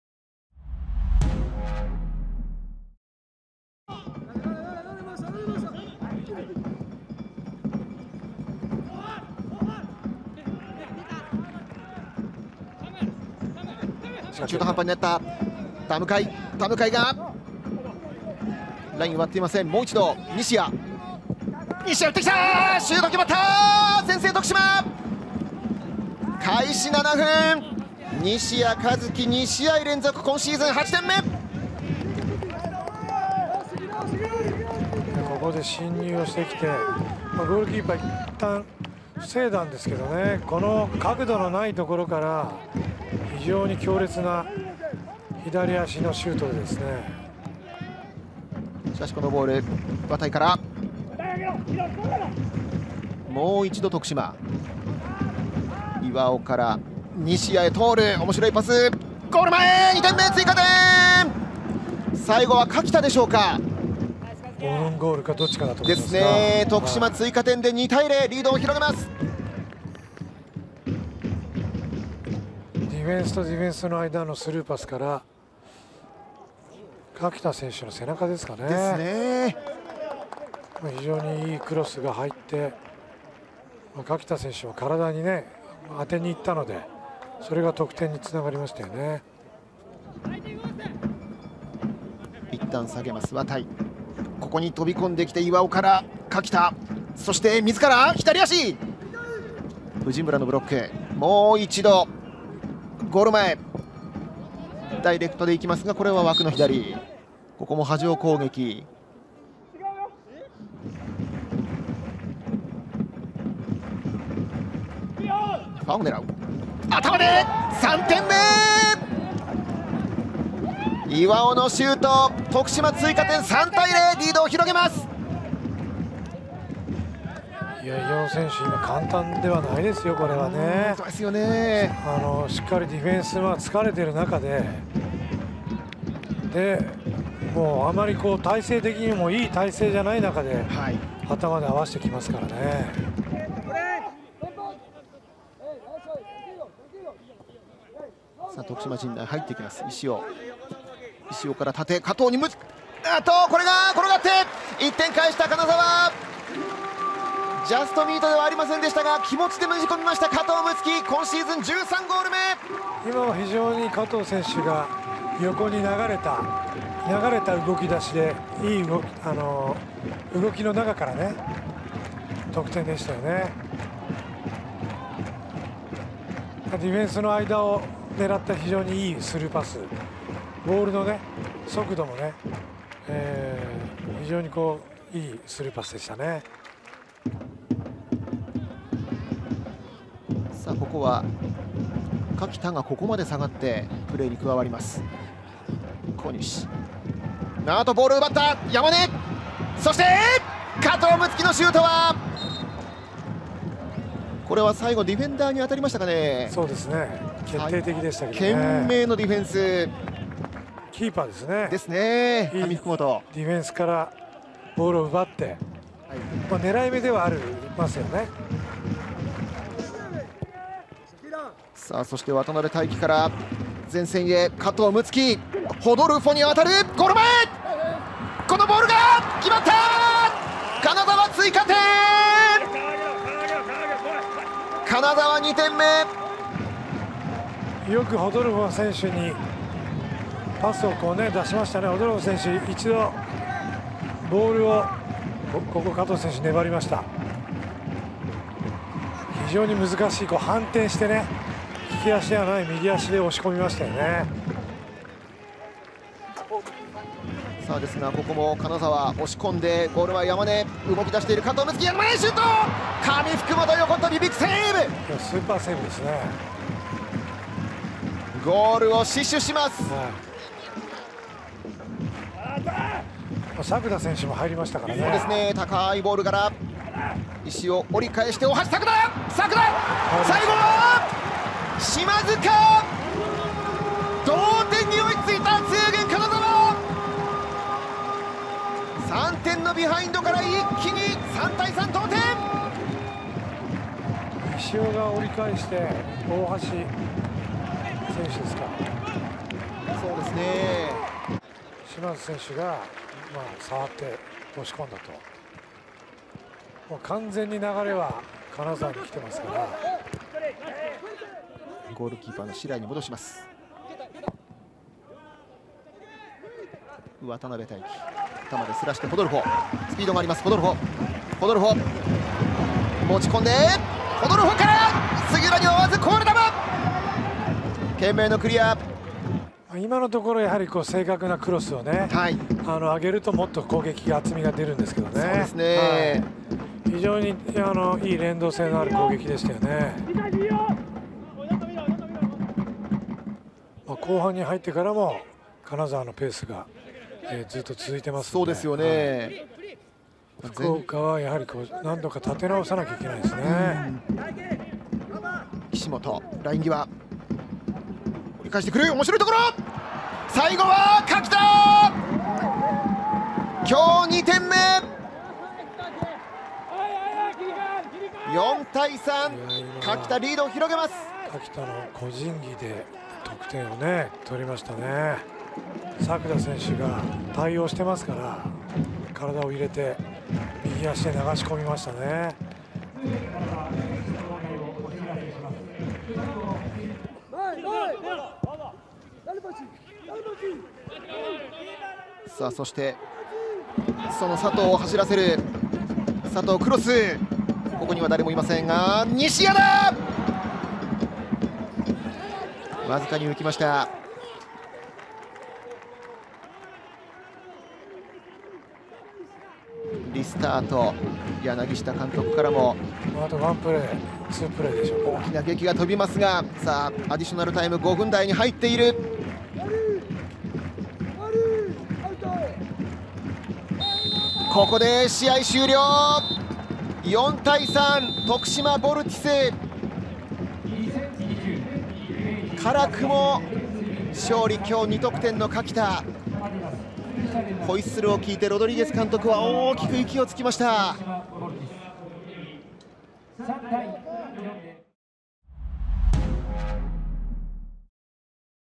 第37節 徳島ヴォルティス vs ツエーゲン金沢
【2020明治安田生命J2リーグハイライト動画配信】

11月29日（日）14:00キックオフ / 石川県西部緑地公園陸上競技場